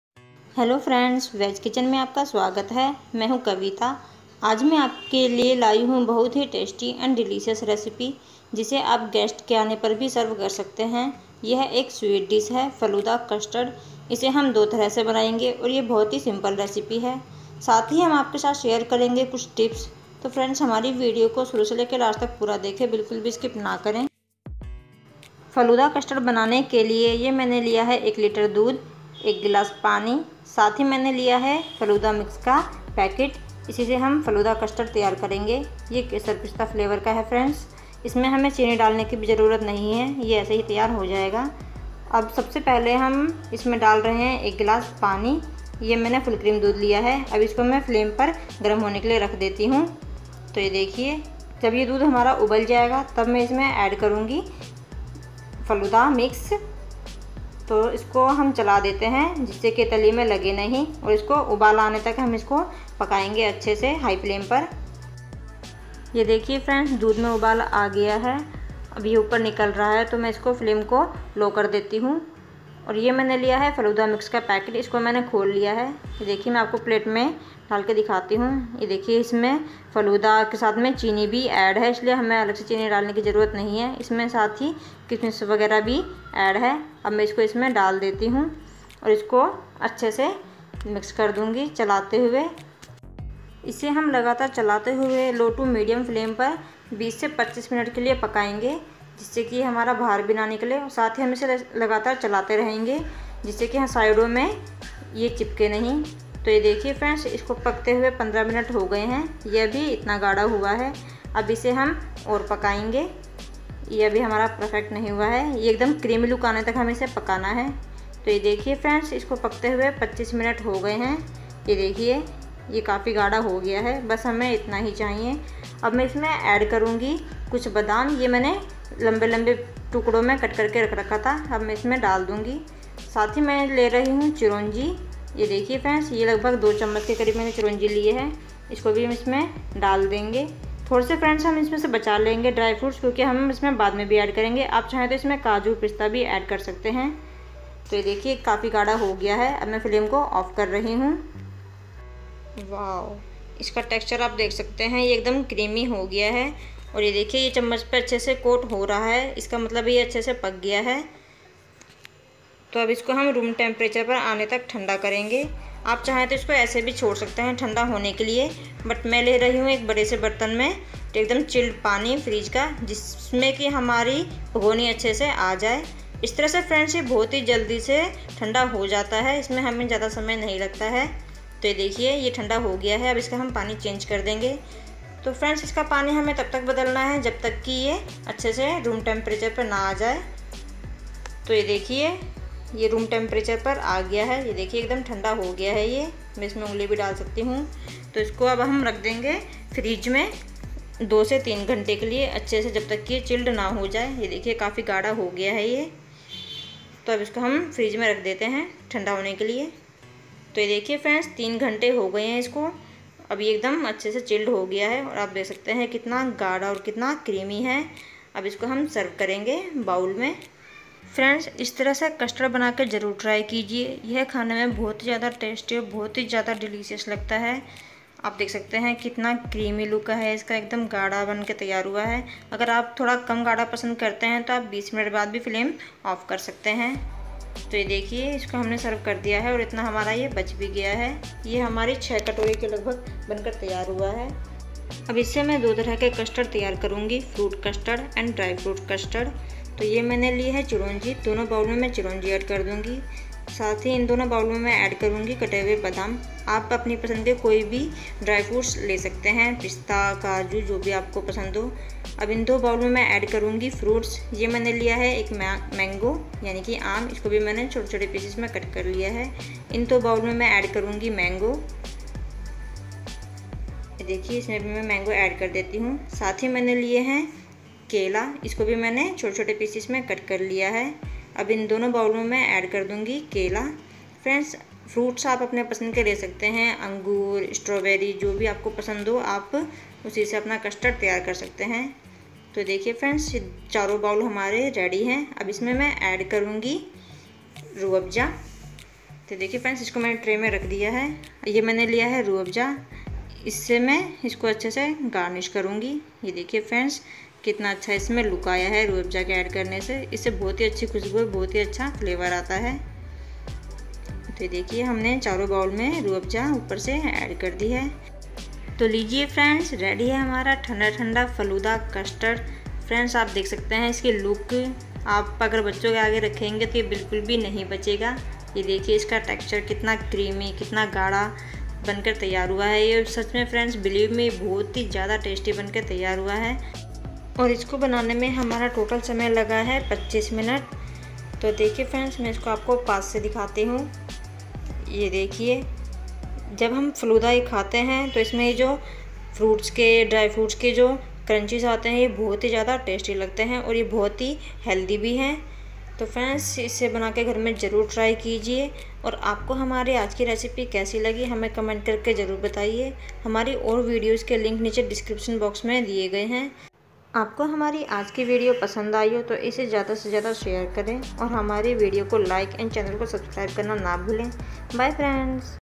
|क्रीमी फलूदा कस्टर्ड/ Fruit Custard/ Dry Fruit Custard/ Custard racipe by "Veg Kitchen" in hindi |
# custard recipe  # falooda custard recipe
# dry fruit custard recipe fruit falooda custard # custard recipe by veg kitchen

I am Kavita

 about this video:

is video mein Ham aapke sath share kar rahe hain 2 in 1 recipe falooda custard, Jise Hamne  do Tarah Se Banaya Hai Dry fruit custard and Fruit custard. Yeh hai ek sweet recipe hai jo bahut jyada tasty delicious Lagti Hai. aur yah Hamare Hath ke liye bhi  faydemand hai.

ingredients:

falooda mix Pista flavour     1 packet
chopped mango                     1
chopped banana                    1
chopped  almond                   2 tsp
chironji                                     2 tsp
rooh afza                                 3 tsp

 हमारी और वीडियोस के लिंक नीचे दिए गए हैं